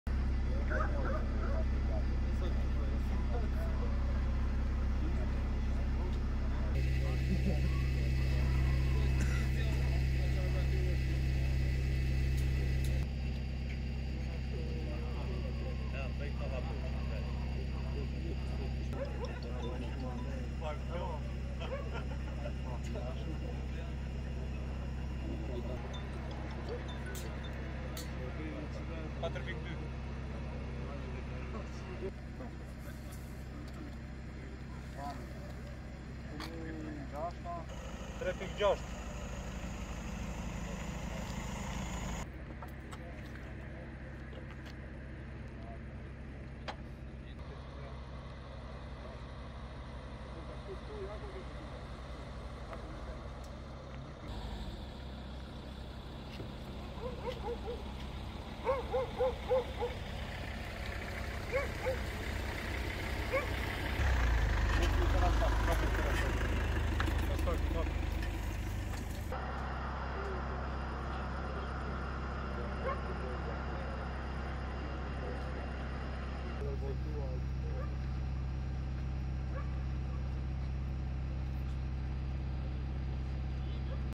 Aksident në aksin rrugor Lezhë-Shkodër, përplasen 2 makina
Një aksident rrugor ka ndodhur në aksin rrugor Lezhë-Shkodër, në Mabë.

Dy mjete janë përplasur me njëri-tjetrin.

Paraprakisht bëhet me dije se janë dëmtuar dy drejtuesit e mjeteve.
Ato janë transportuar drejt spitalit Rajonal të Lezhës.
Policia është nisur drejtë vendngjarjes.